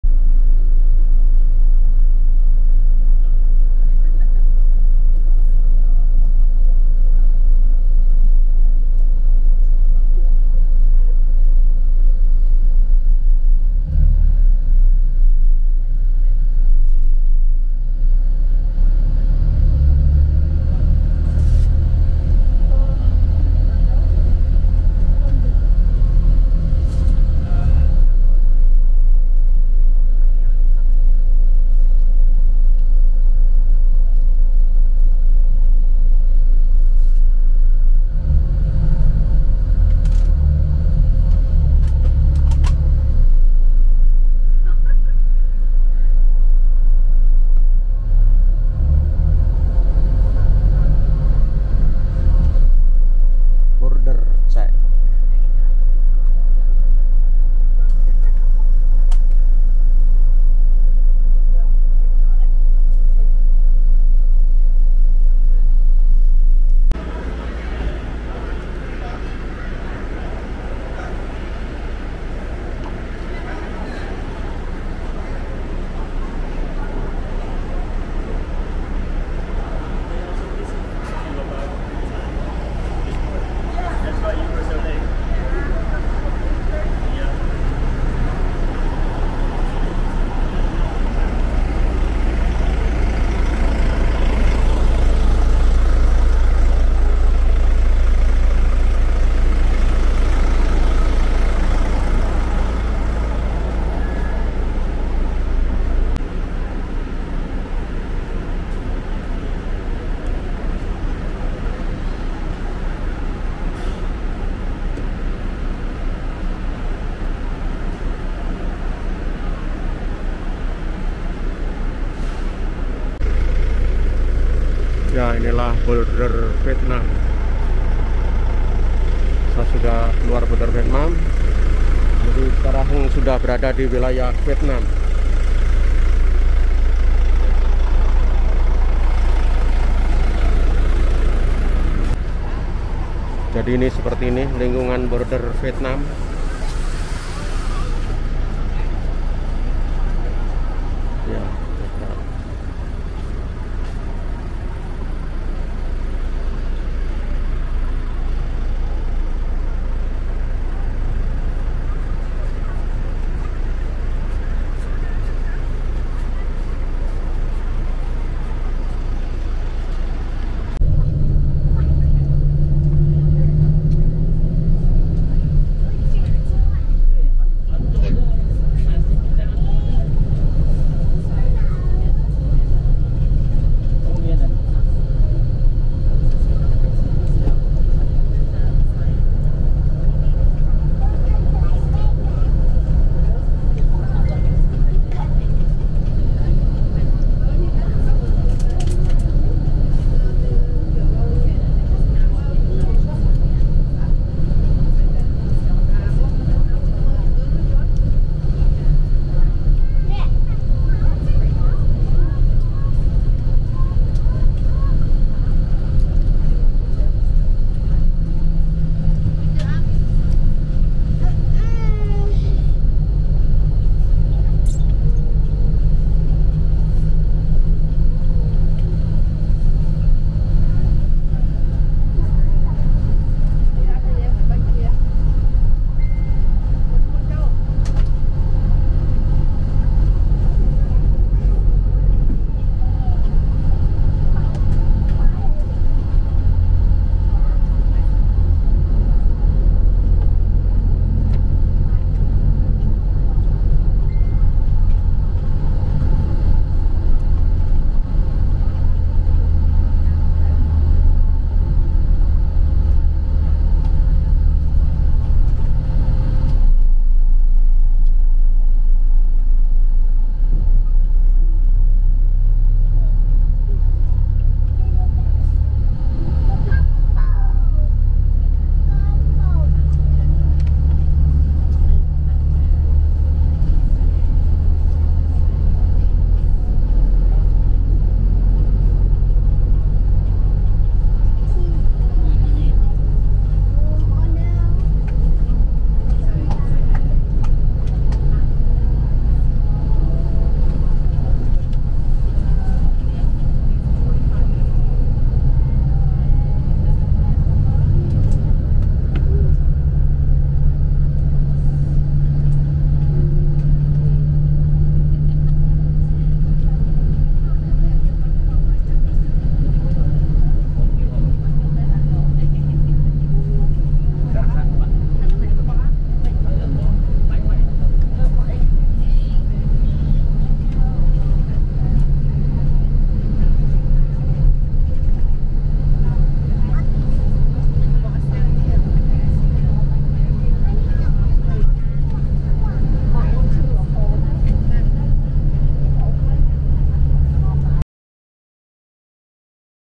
Solo Traveling 5 negara , tiba di Border Bavet Cambodia And Moc Bai Vietnam
Solo Traveling 5 negara
Malintasi Perbatasan Kamboja dan Vietnam lewat Border Bavet dan Moc Bai
Border Bavet Cambodia And Moc Bai Vietnam.

Border Cambodia Vietnam, Immigration Processing, bavet border, border vietnam cambodia,
Elm Family Indonesia, Elm Family, Elm Putri, Elm Channel, Elm,